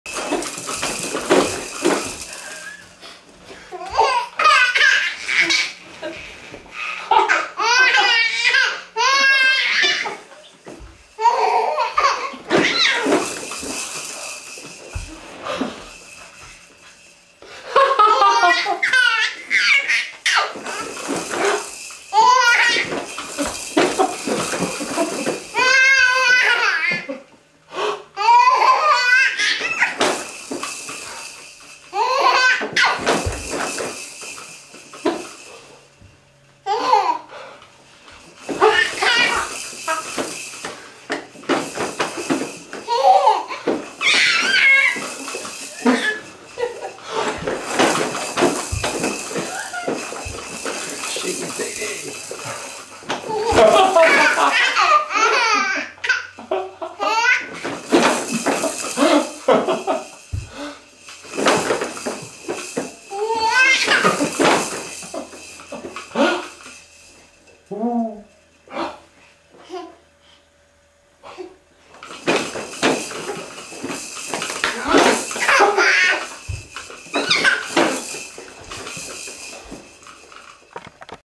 Robin won our laughing contest
Robin just turned 6 month yesterday and he and his father decided to do a laughing contest. Actually, it was not much of a contest. He clearly won :)